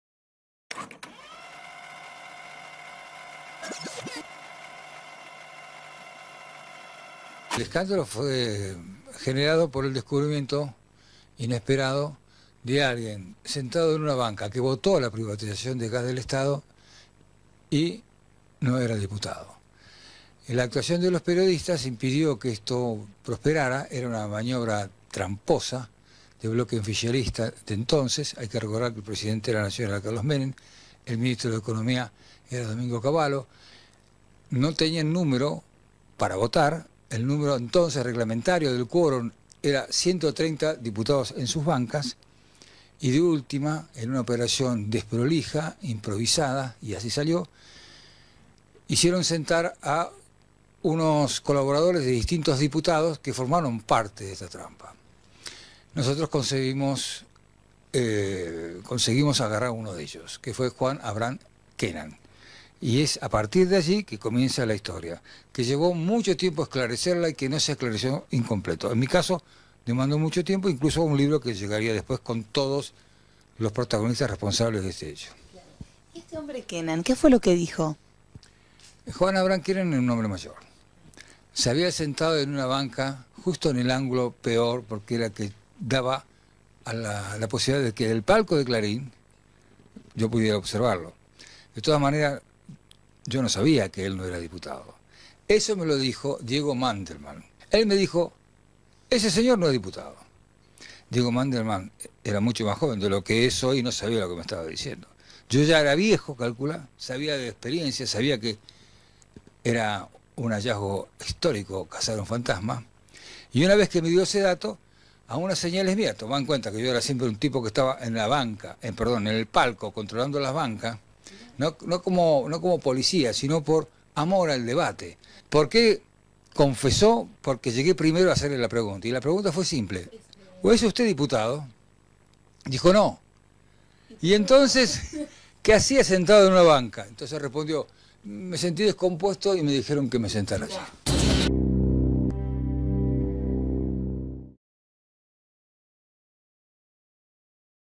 ARMANDO VIDAL EL PERIODISTA QUE DESCUBRIÓ EL DIPUTRUCHO
En la Cámara de Diputados se debatía la ley de privatización de Gas del Estado, clave para el menemismo. Los radicales se retiraron del recinto y el justicialismo tuvo que esforzarse para obtener quórum. Tanto que hasta consiguieron a un intruso que se sentó en una banca y votó a favor de la ley. Así nació el escándalo del “diputrucho”.
El 26 de mayo de 1992, Juan Abraham Kenan, un asesor de 72 años del diputado peronista Julio Manuel Samid, entró al recinto y simuló ser diputado para que se apruebe el marco regulatorio para la privatización del gas.
El caso fue descubierto por Armando Vidal, periodista de Clarín , que presenciaba la sesión desde un palco y detectó al intruso.
Vidal persiguió a Kenan cuando se fue del recinto y logró interceptarlo antes de que saliera. Entonces le preguntó a Kenan si era diputado: “No”, confesó Kenan, “pero como no me sentía bien me dijeron que me sentara ahí un rato”.
Según Vidal, en la votación habrían participado otros cinco intrusos , todos ellos colaboradores de diputados justicialistas. A diferencia de Kenan, lograron escapar del recinto antes de ser identificados.
Una vez acorralado, Kenan sufrió una descompensación y fue asistido, pero igual pasó la noche en una comisaría, detenido .
Aunque se trató de un escándalo mediático, la Cámara de Diputados no sancionó al diputado Samid por el hecho, ni tampoco al presidente de la Cámara, Alberto Pierri, que validó la irregular sesión.
La votación debió realizarse nuevamente al poco tiempo. En 1994, Samid y Kenan fueron condenados a varios meses de prisión en suspenso .